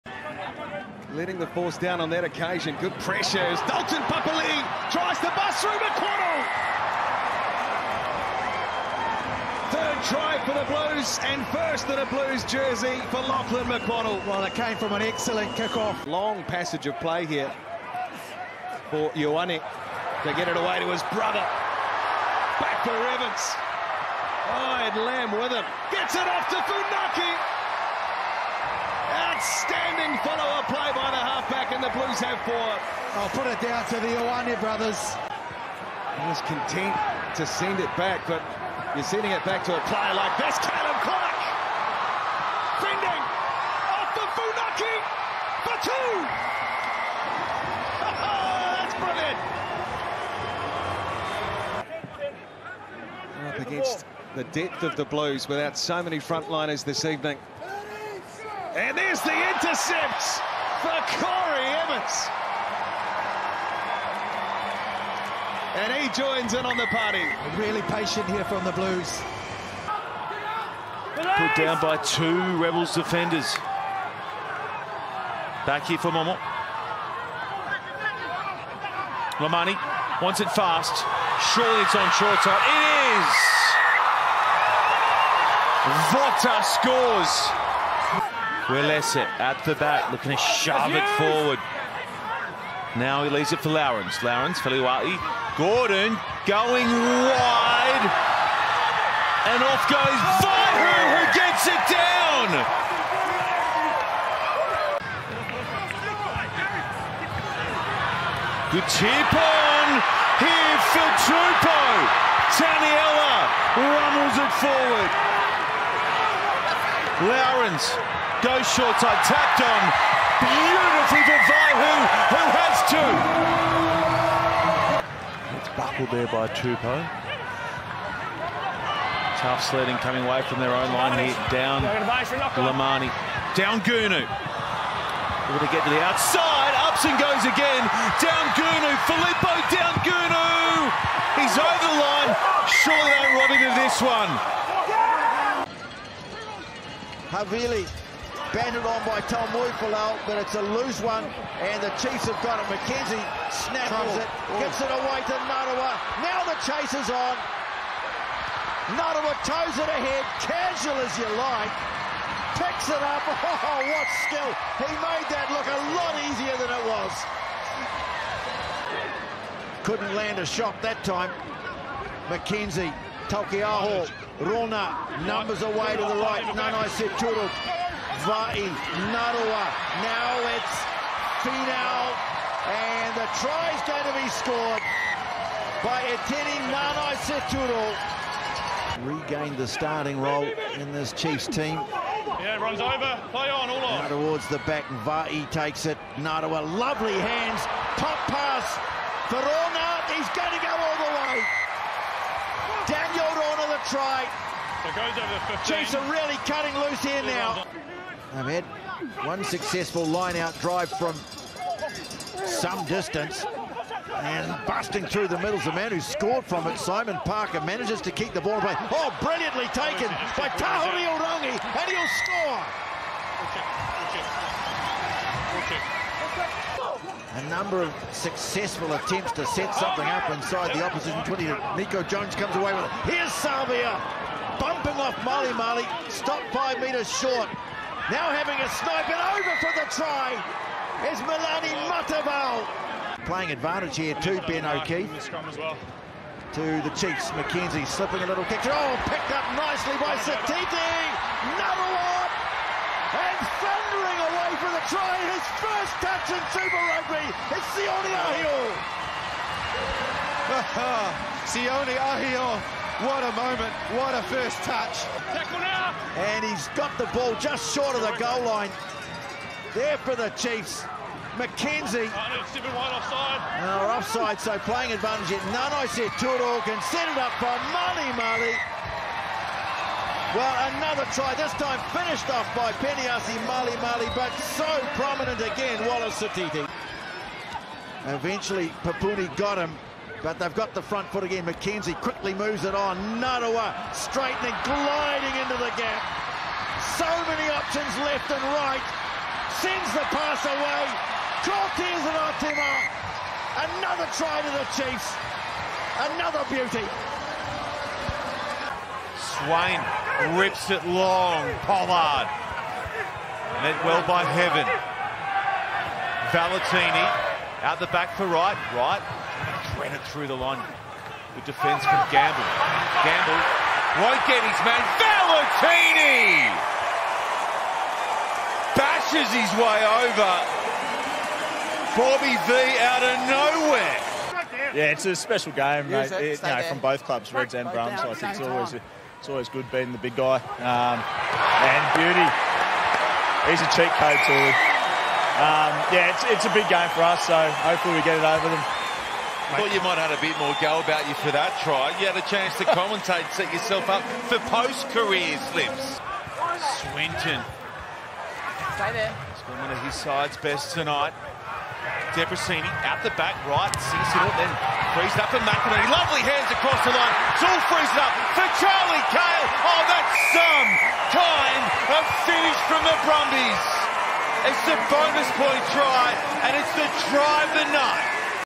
The Tries of Round 7 | Super Rugby Pacific 2024
The top tries from Round 7 of Super Rugby Pacific 2024!